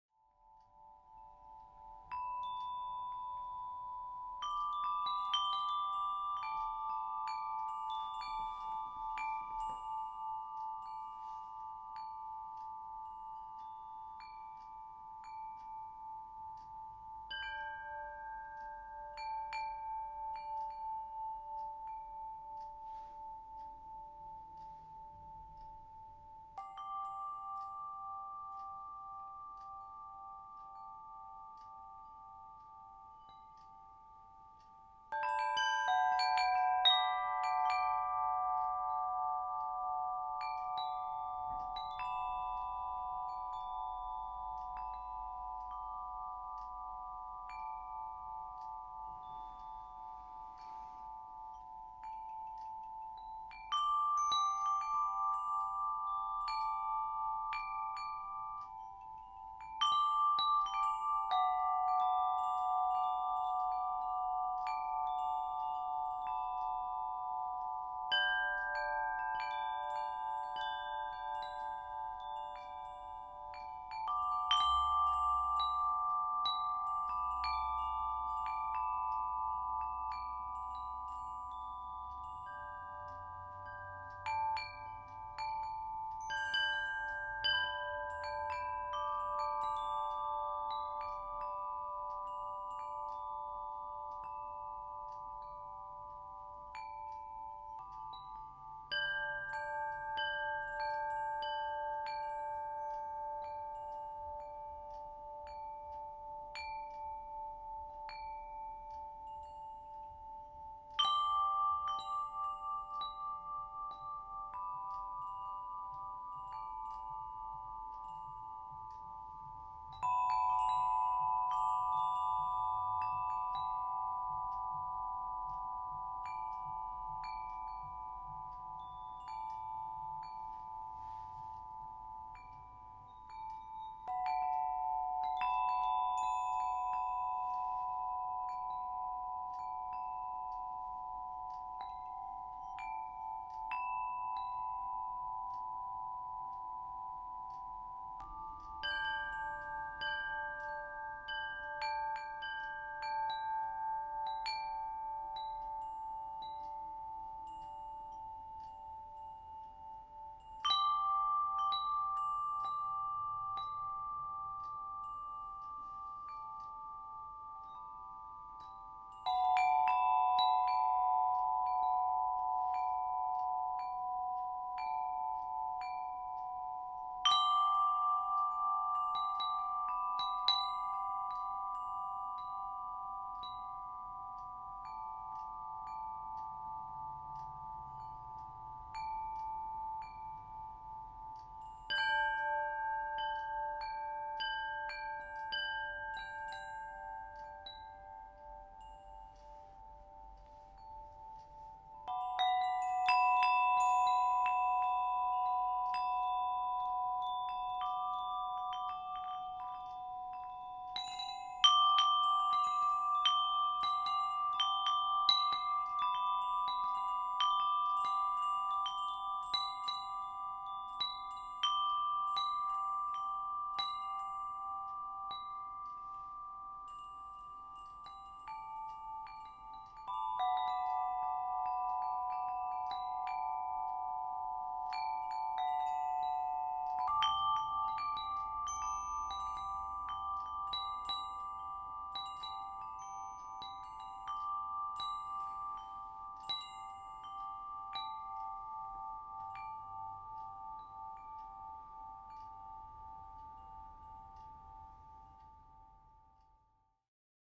Pachelbel Chimes & Snow
Got this Patchelbel set of chimes for Xmas. Fell in love with the sound as soon as I heard it. And thought, "There's a good video to be made around these sounds." And there was. A day later, snow arrived. Out came the camera. And after a few days of learning how to record of the sounds of the chimes.(And some serious editing of both the chimes and the video.) Out came this. I like it. Lots. Hope you do, too